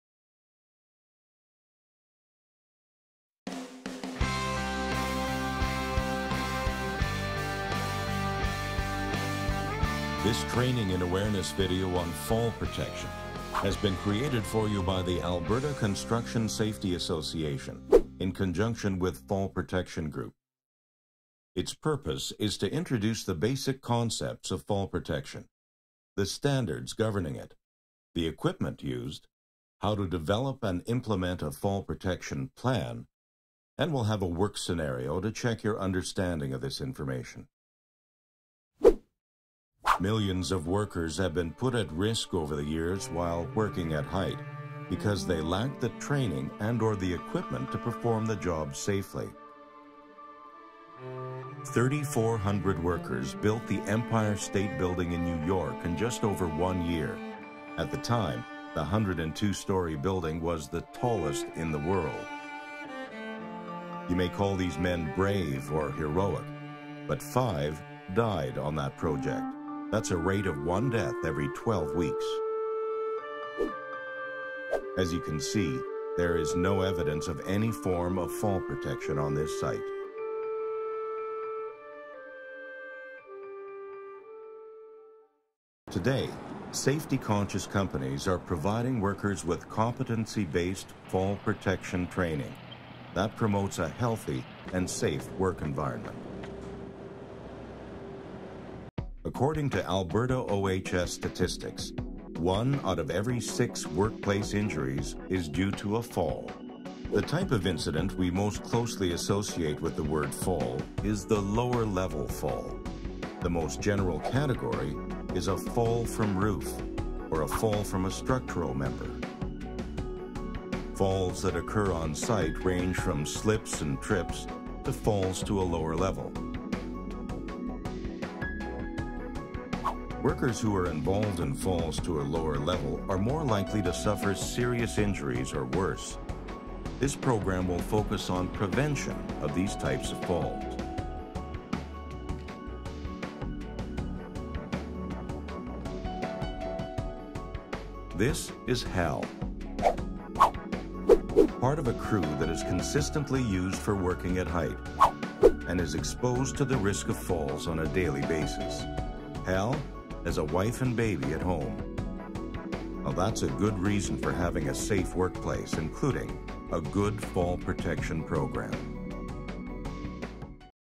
Fall Protection: Training and Awareness Module 1 | Your ACSA Safety Training
Millions of workers have been put at risk over the years while working at heights because they lack the training and/or equipment to perform the job safely. This training and awareness video on fall protection will introduce the basic concepts of fall protection, the standards governing it, the equipment used, how to develop and implement a fall protection plan, and a work scenario to check your understanding of this information.